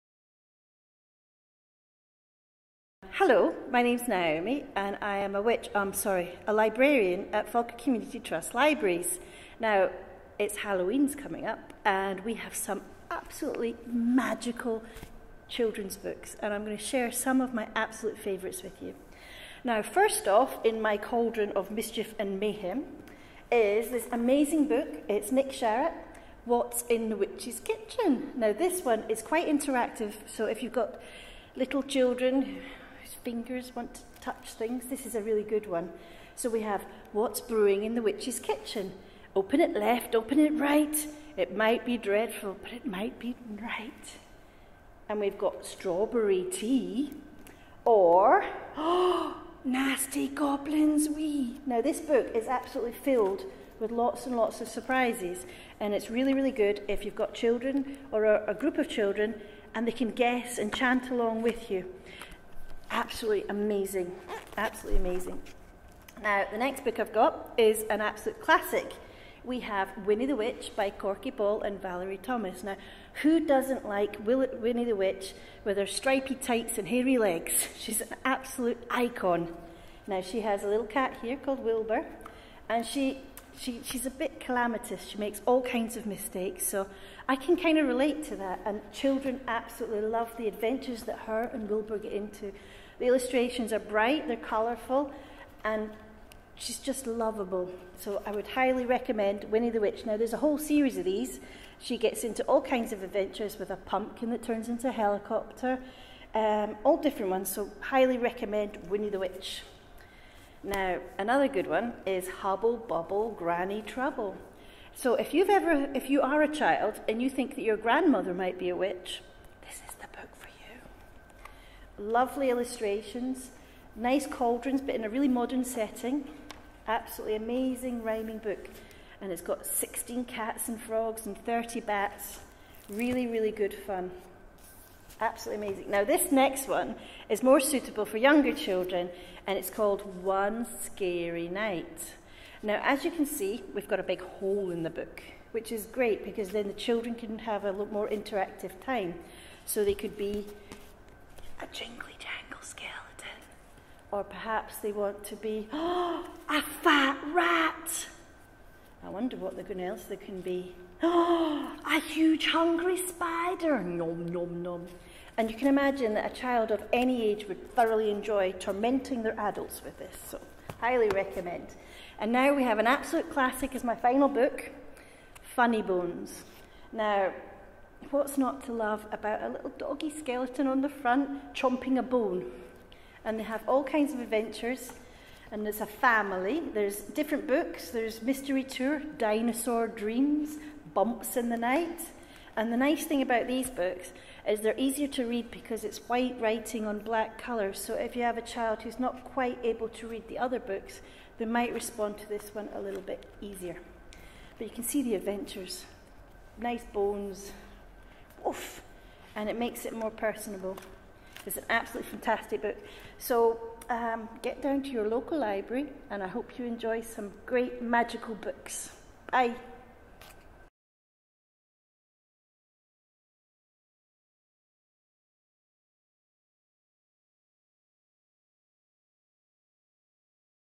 Library Love Halloween children's books
A cauldron of magical children's books suitable for a spooky Hallowe'en storytelling!